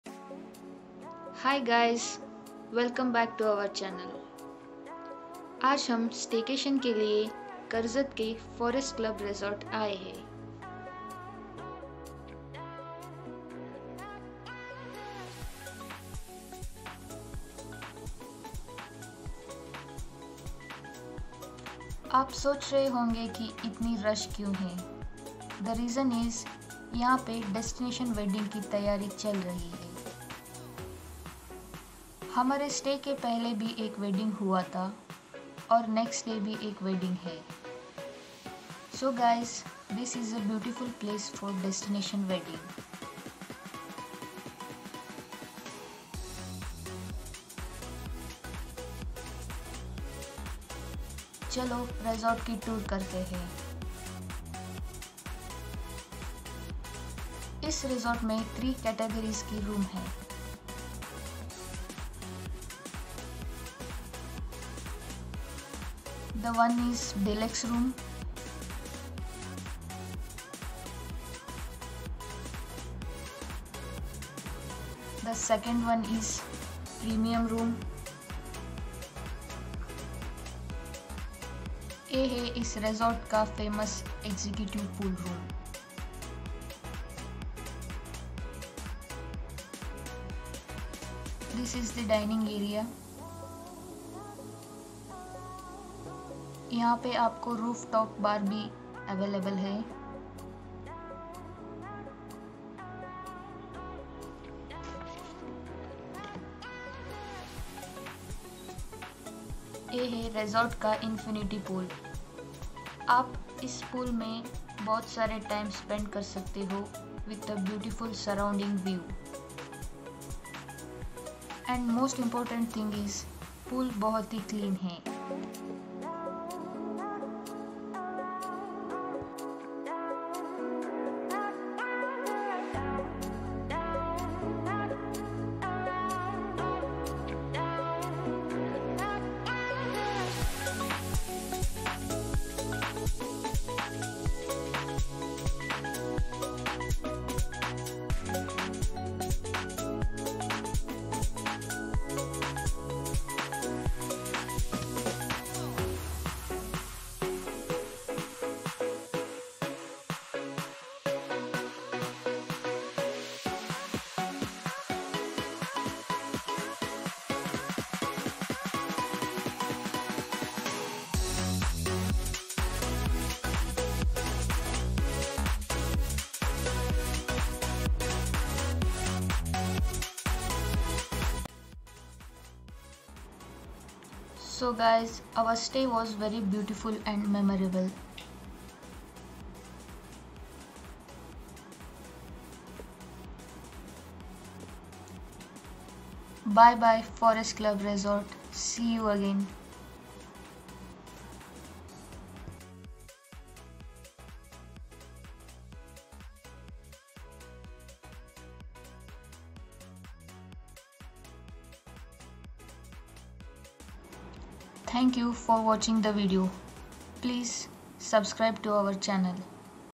The Forest Club Resort, Karjat | Staycation
The Forest Club Resort is a gorgeous property in Karjat bringing all your Staycation fantasies come true.